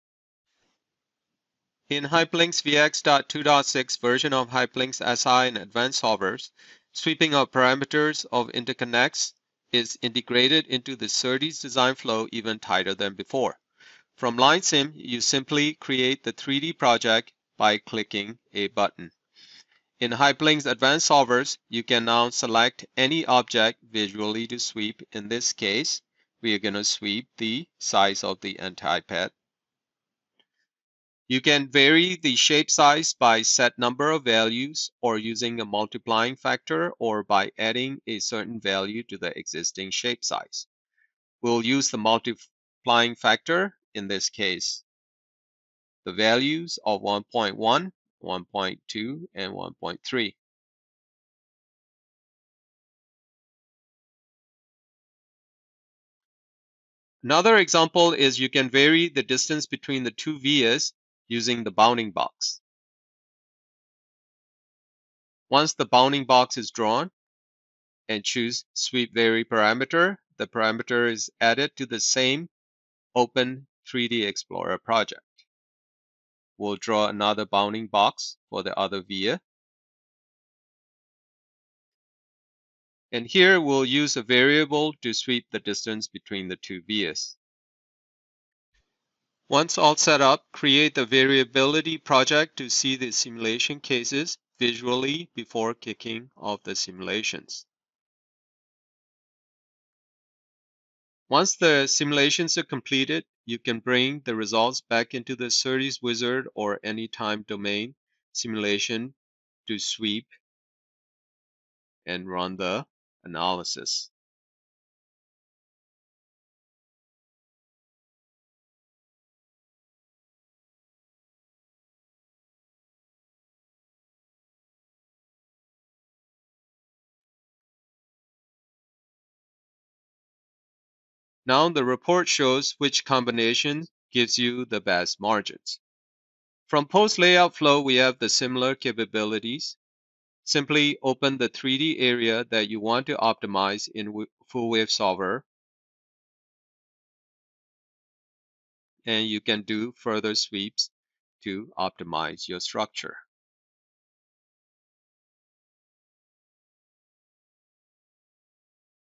Optimize Interconnect with 3D EM
HyperLynx VX.2.6. HyperLynx 3D Explorer lets you sweep design geometries and model behavior through 3D EM simulation to create a structure that meets your design’s electrical performance needs. Optimize via transitions, component fanout, blocking capacitor layout and more. 3D Explorer lets you start from a library of preconfigured templates or create your own projects from PCB layout.